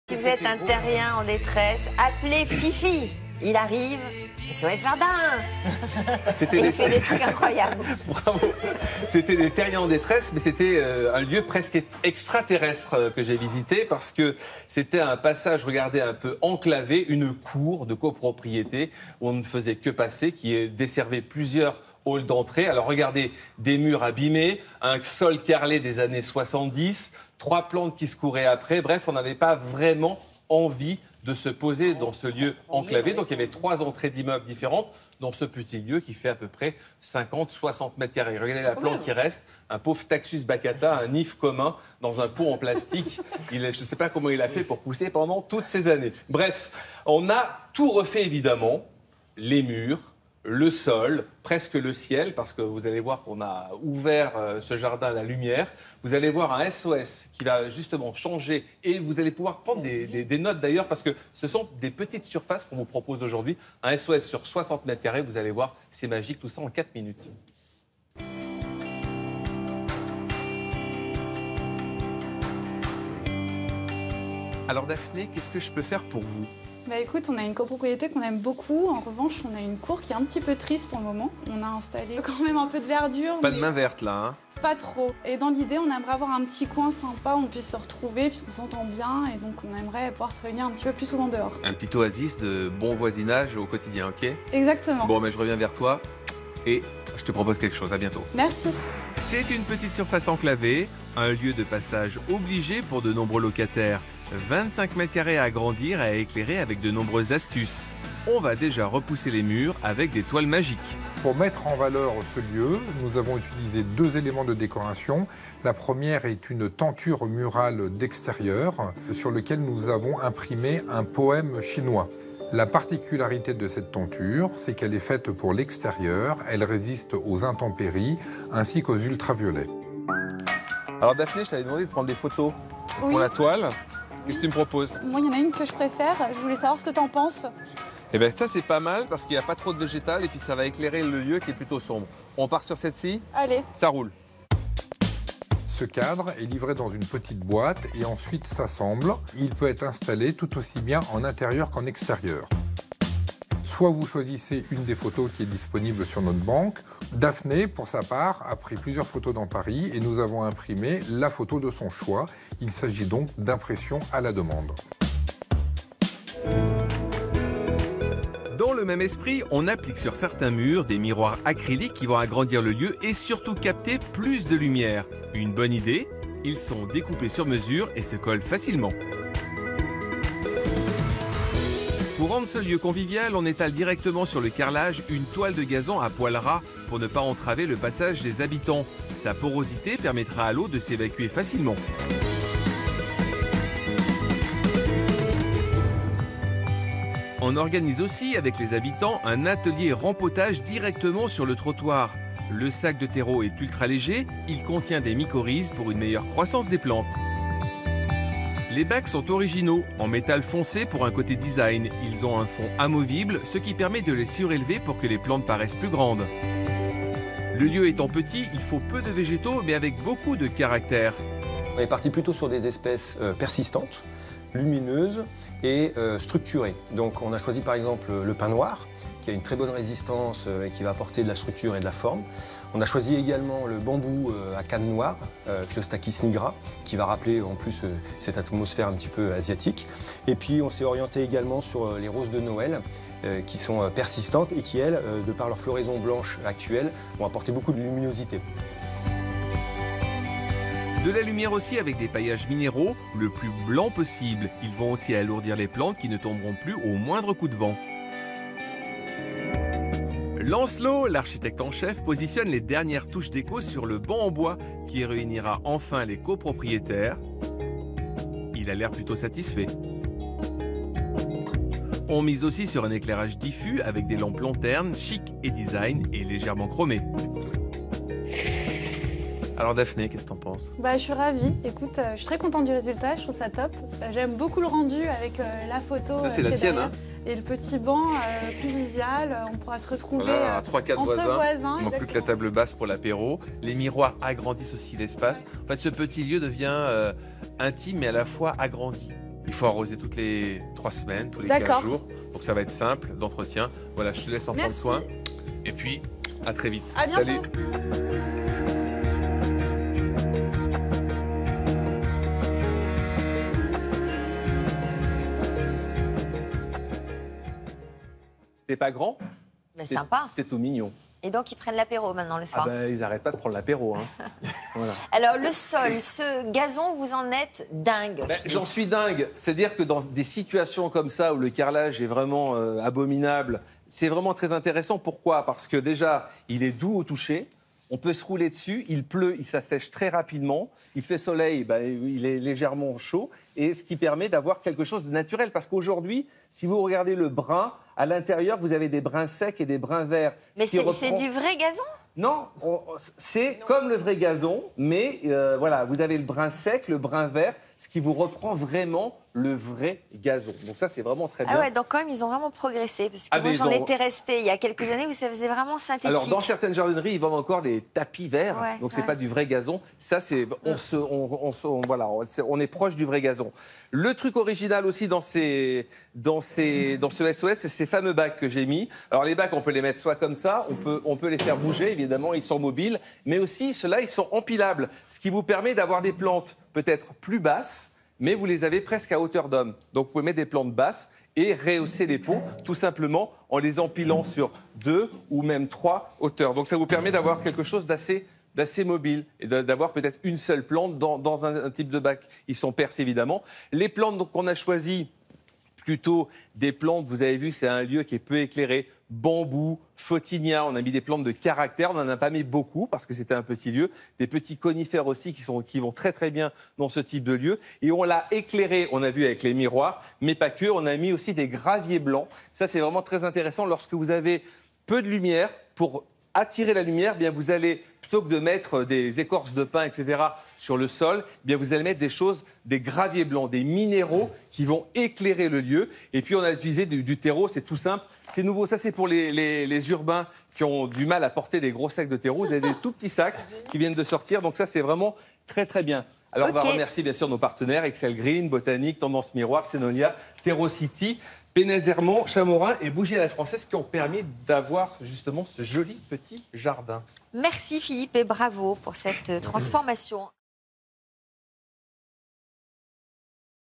JardiCube et Gravier Blanc - C'est au programme - 20170407
Retrouvez nos pots de fleurs empilables JARDICUBE et notre Gravier Blanc Minériale !